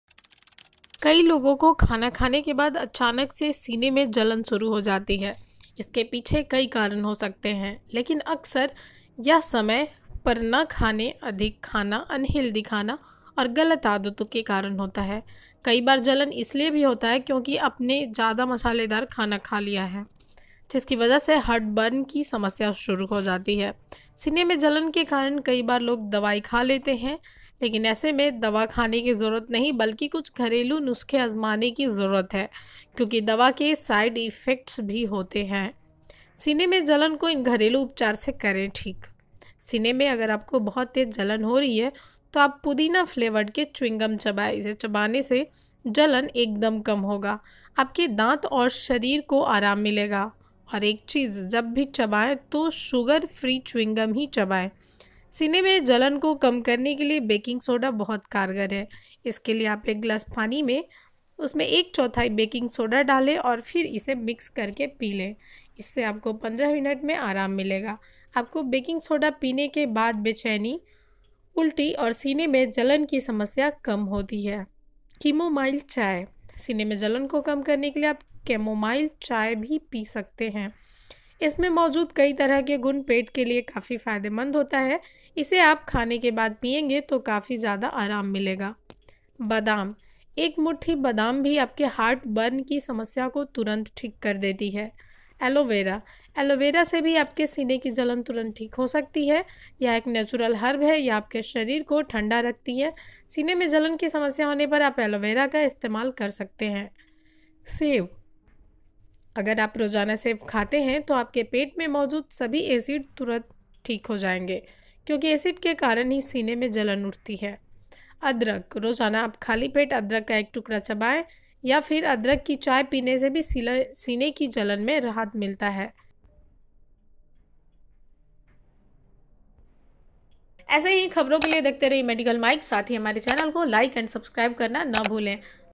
खाने के तुरंत बाद सीने में रहती है जलन, काम आएंगे ये नुस्खे।
खाने के तुरंत बाद सीने में रहती है जलन, काम आएंगे ये नुस्खे। कई लोगों को खाना खाने के बाद अचानक से सीने में जलन शुरू हो जाती है। इसके पीछे कई कारण हो सकते हैं। अक्सर यह समय पर न खाने, अधिक खाना, अनहेल्दी खाना और गलत आदतों के कारण होता है। कई बार जलन इसलिए भी होता है क्योंकि आपने ज्यादा मसालेदार खाना खा लिया है। जिसकी वजह से हर्ट बर्न की समस्या शुरू हो जाती है। सीने में जलन को इन घरेलू उपचार से करें ठीक। सीने में अगर आपको बहुत तेज जलन हो रही है तो आप पुदीना फ्लेवर्ड के चुइंगम चबाएं। सीने में जलन को कम करने के लिए आप कैमोमाइल चाल भी पी सकते हैं। एक मुट्ठी बादाम भी आपके हार्ट बर्न की समस्या को तुरंत ठीक कर देती है। एलोवेरा से भी आपके सीने की जलन तुरंत ठीक हो सकती है। यह एक नेचुरल हर्ब है। अगर आप रोजाना सेब खाते हैं तो आपके पेट में मौजूद सभी एसिड तुरंत ठीक हो जाएंगे। रोजाना आप खाली पेट अदरक का एक टुकड़ा चबाने या फिर अदरक की चाय पीने से भी सीने की जलन में राहत मिलता है।